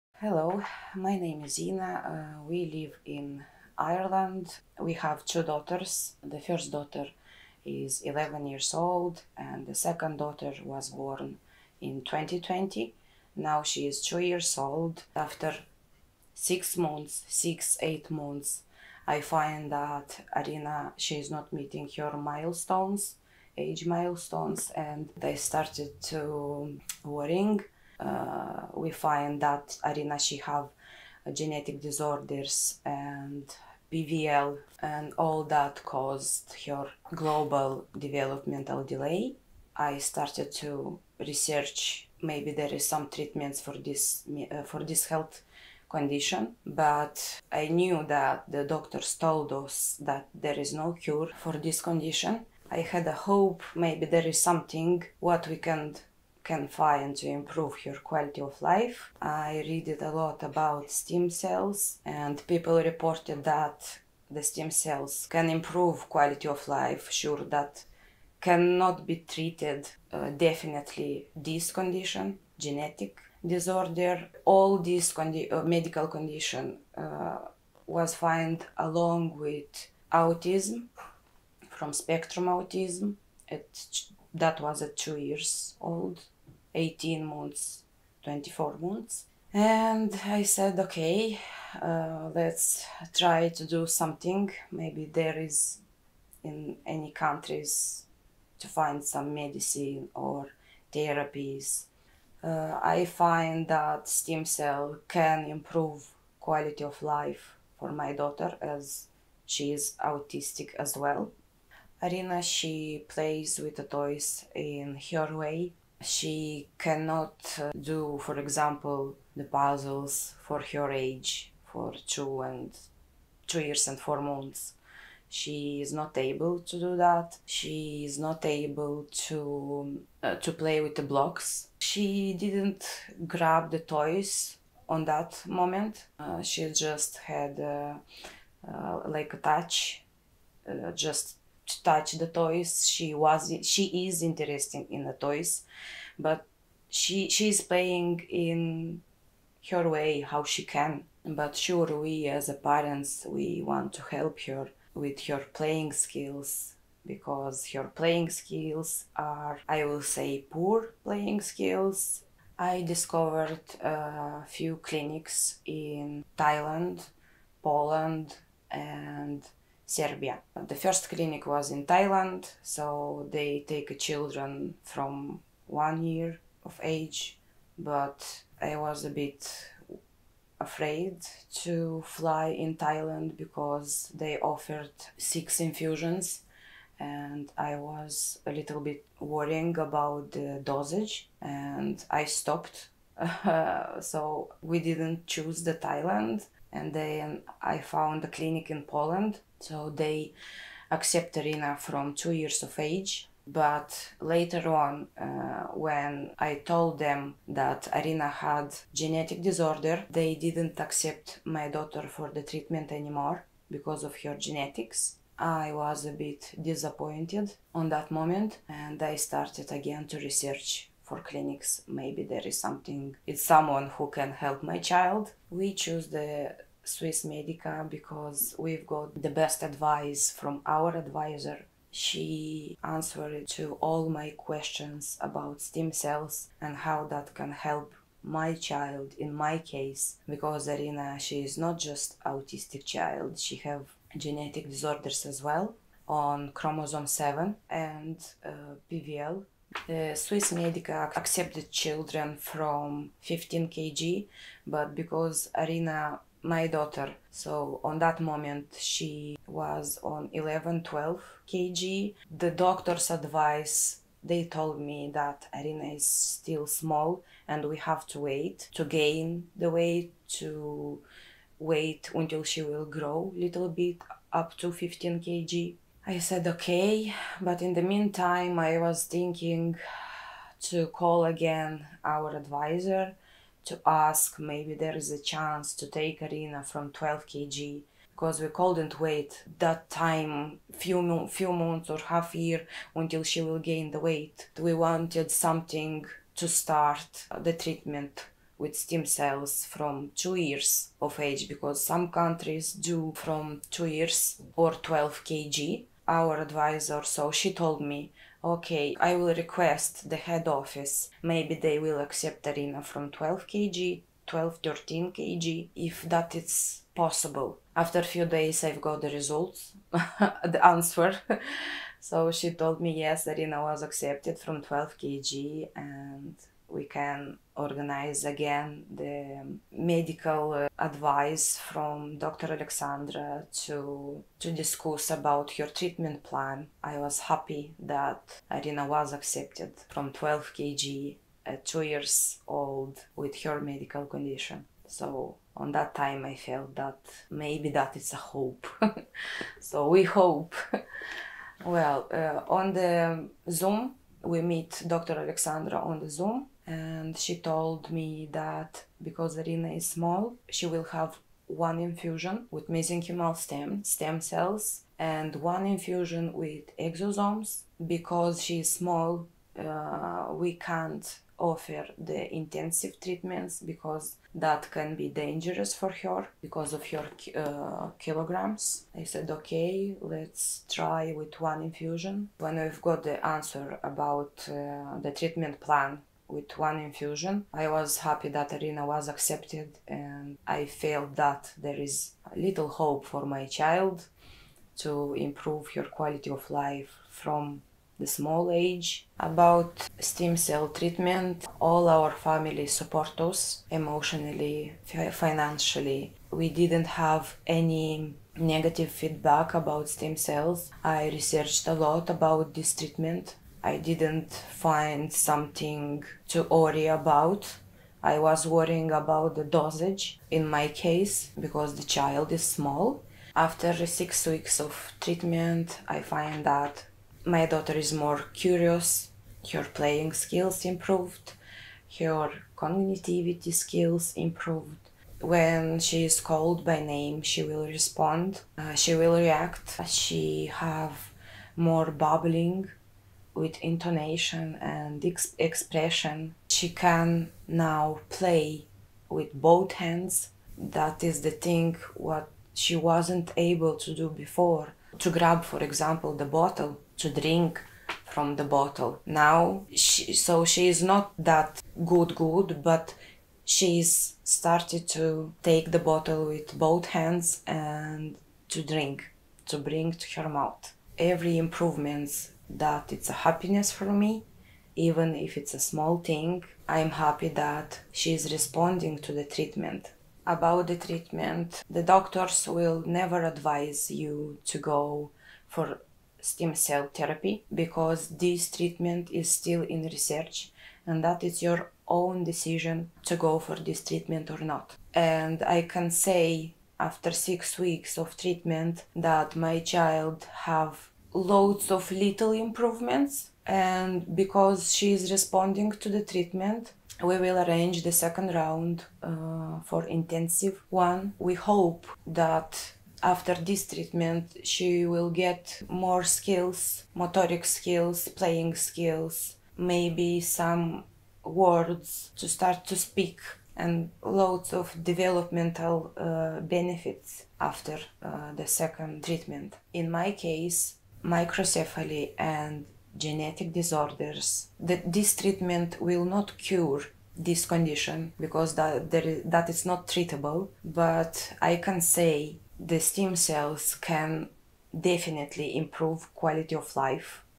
One Family's Journey with Stem Cell Therapy for Autism and Genetic Disorders
To find out more about stem cells for autism, visit the

Hope and Progress: One Family's Journey with Stem Cell Therapy for Autism and Genetic Disorders

Ina, a mother from Ireland, was determined to find a way to improve her daughter's quality of life. Her daughter was born with genetic disorders and global developmental delay, as well as being on the autism spectrum. Despite being told by doctors that there was no cure for her daughter's condition, Ina refused to give up hope and began researching alternative treatments.

Ina eventually found stem cell therapy. Although stem cell therapy is not a cure for her daughter's condition, she found hope in the possibility of improvement.

In the video, Ina shares her journey to finding the right clinic for her daughter's stem cell therapy, and the improvements that she's seen after stem cell therapy. From improved playing skills to increased curiosity and communication.

If you're a parent searching for hope and a way to improve your child's quality of life, watch our video and learn more about Ina's experience with stem cell therapy.

*𝘗𝘭𝘦𝘢𝘴𝘦 𝘯𝘰𝘵𝘦: 𝘛𝘩𝘦 𝘤𝘰𝘯𝘵𝘦𝘯𝘵 𝘱𝘳𝘰𝘷𝘪𝘥𝘦𝘥 𝘪𝘯 𝘵𝘩𝘪𝘴 𝘷𝘪𝘥𝘦𝘰 𝘪𝘴 𝘯𝘰𝘵 𝘪𝘯𝘵𝘦𝘯𝘥𝘦𝘥 𝘢𝘴 𝘮𝘦𝘥𝘪𝘤𝘢𝘭 𝘢𝘥𝘷𝘪𝘤𝘦. 𝘐𝘯𝘥𝘪𝘷𝘪𝘥𝘶𝘢𝘭 𝘵𝘳𝘦𝘢𝘵𝘮𝘦𝘯𝘵 𝘳𝘦𝘴𝘶𝘭𝘵𝘴 𝘮𝘢𝘺 𝘷𝘢𝘳𝘺. 𝘈𝘭𝘸𝘢𝘺𝘴 𝘤𝘰𝘯𝘴𝘶𝘭𝘵 𝘸𝘪𝘵𝘩 𝘢 𝘲𝘶𝘢𝘭𝘪𝘧𝘪𝘦𝘥 𝘩𝘦𝘢𝘭𝘵𝘩𝘤𝘢𝘳𝘦 𝘱𝘳𝘰𝘧𝘦𝘴𝘴𝘪𝘰𝘯𝘢𝘭 𝘳𝘦𝘨𝘢𝘳𝘥𝘪𝘯𝘨 𝘢𝘯𝘺 𝘮𝘦𝘥𝘪𝘤𝘢𝘭 𝘤𝘰𝘯𝘥𝘪𝘵𝘪𝘰𝘯.